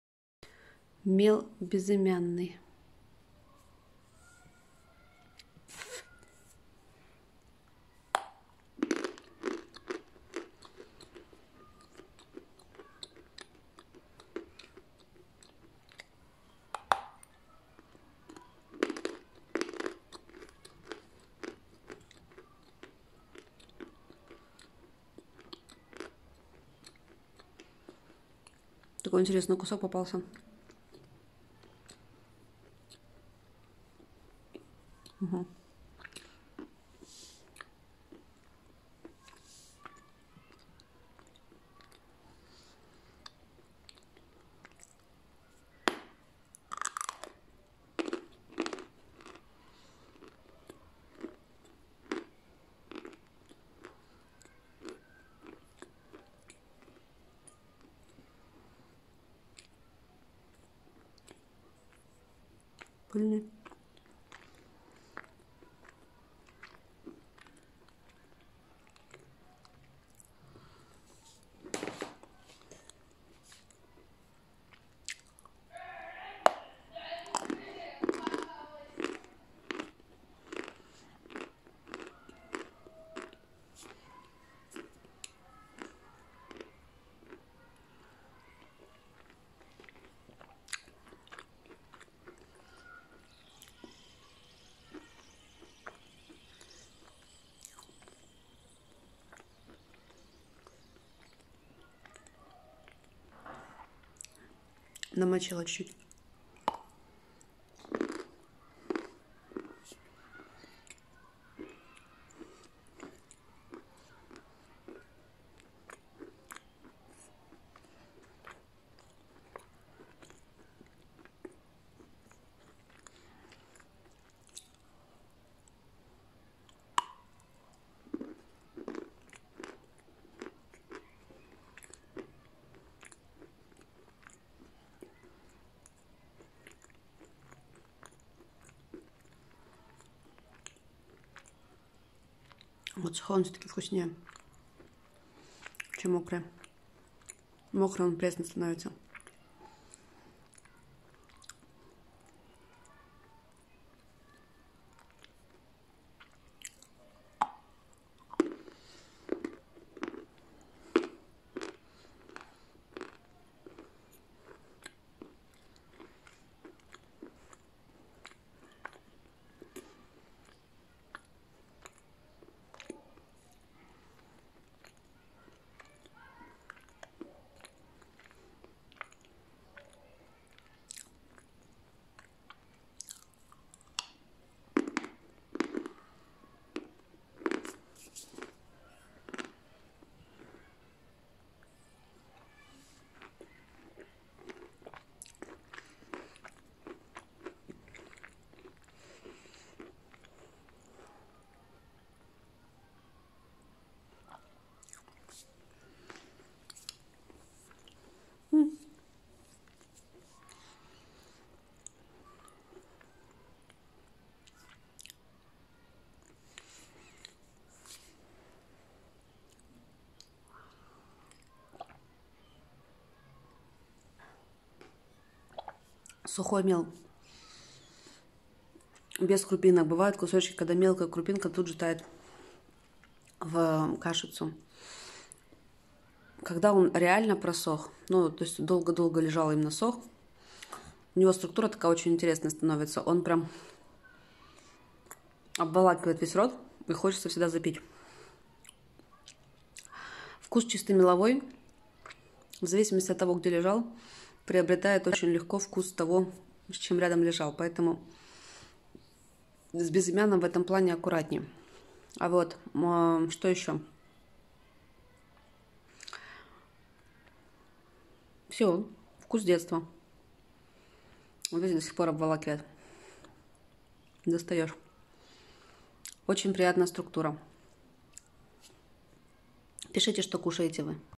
ASMR - Мел Безымянный АСМР chalk crushing,asmr chalk shaving,ASMR chalk cutting eating chalk
Это мега вкусно. Ох уж эти сербанья в конце
В середине рассказываю чем увлекаюсь. В конце чищу глыбы побольше❤️❤️❤️

Не спамьте в комментариях.
За несоблюдение перманентный БАН

Канал Nuni YT.